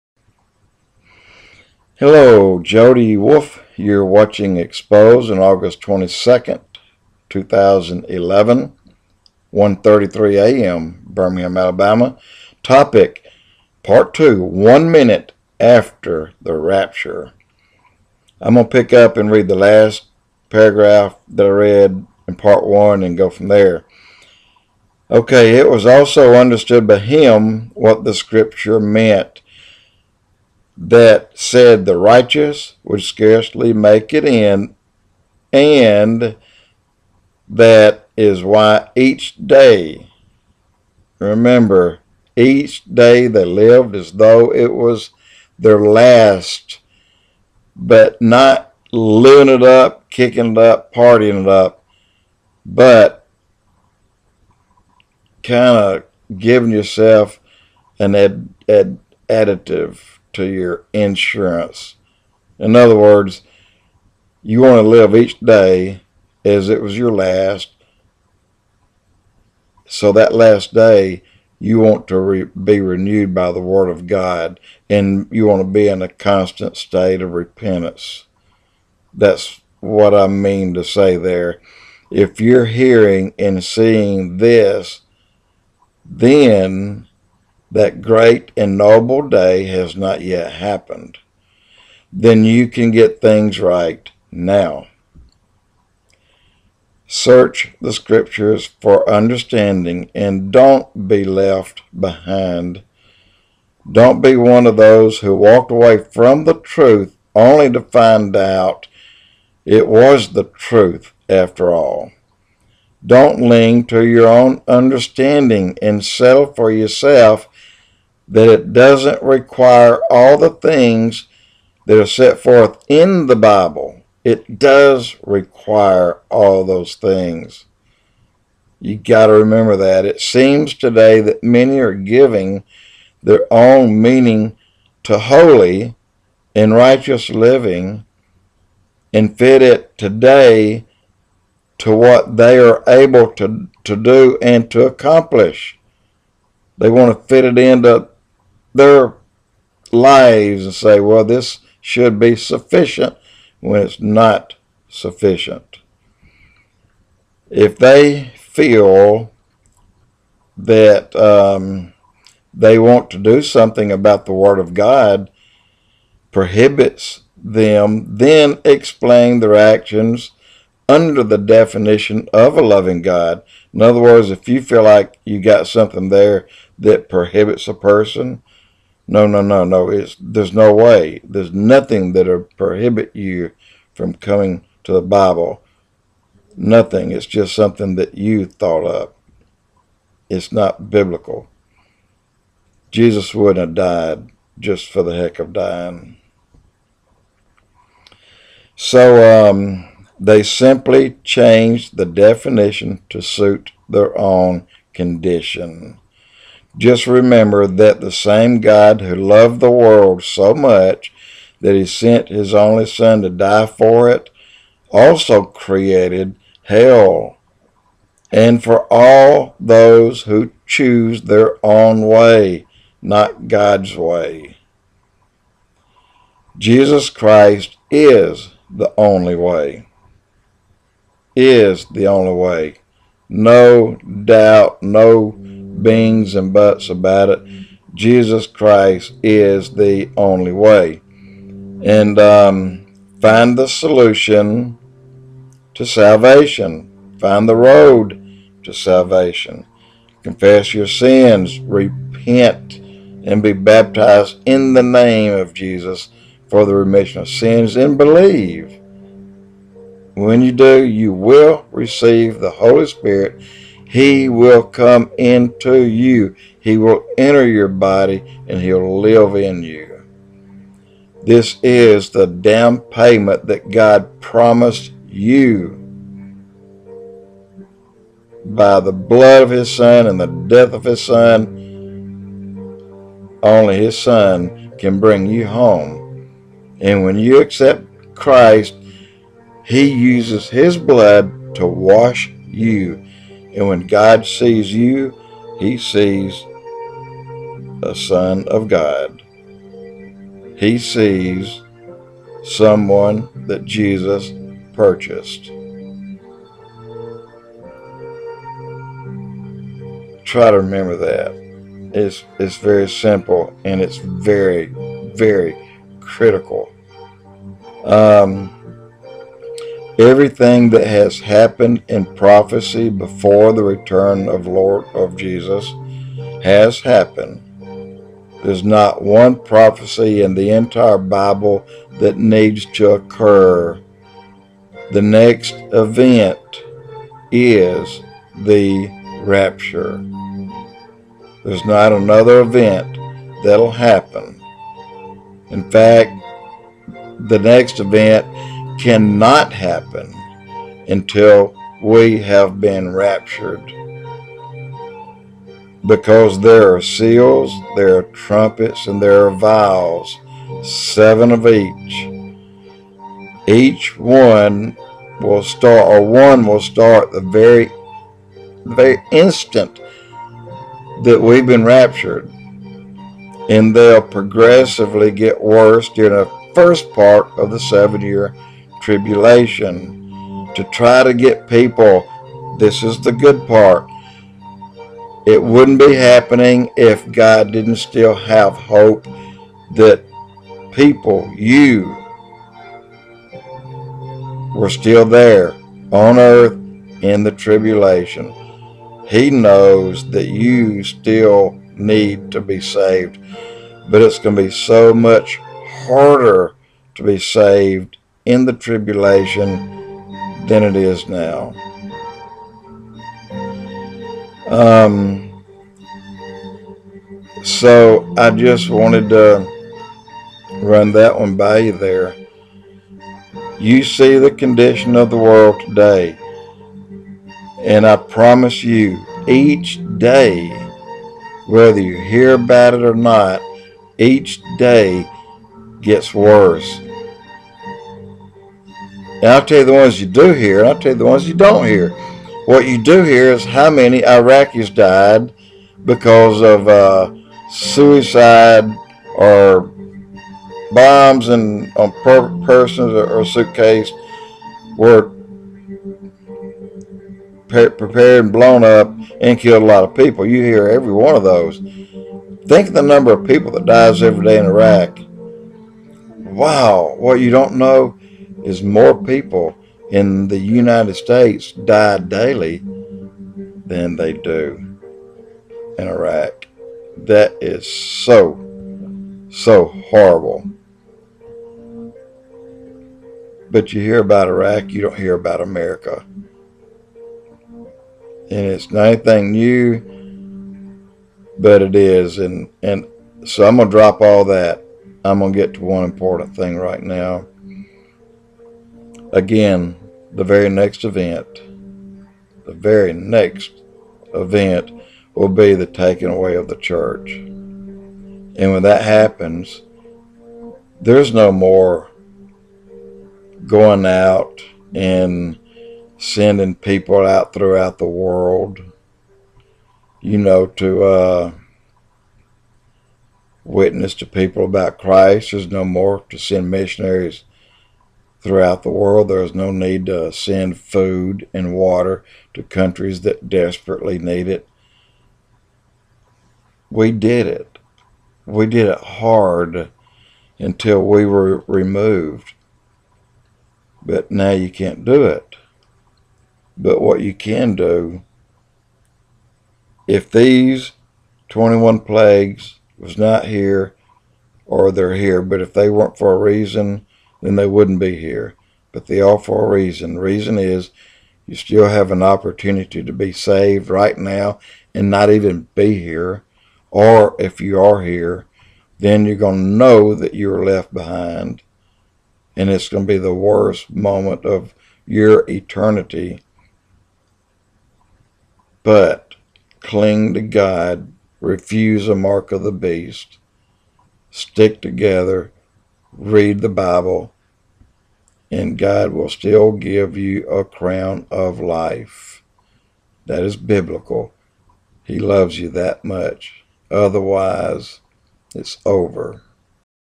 EXPOZ:...One Minute After The Rapture Pt. 2 of 2
One minute later "You Will Know" you have "Been Left Behind".
I honestly do believe since The Tribulation will have 21 disasters such as, famine, sickness, horrible things that will come in wave after wave means one thing, you may have missed the Rapture but your soul can still be saved. Otherwise, God would simply cast everyone into hell. The events are disigned to bring those who want to be saved, into Heaven,
What can I say, This Is Going To Soon Happen.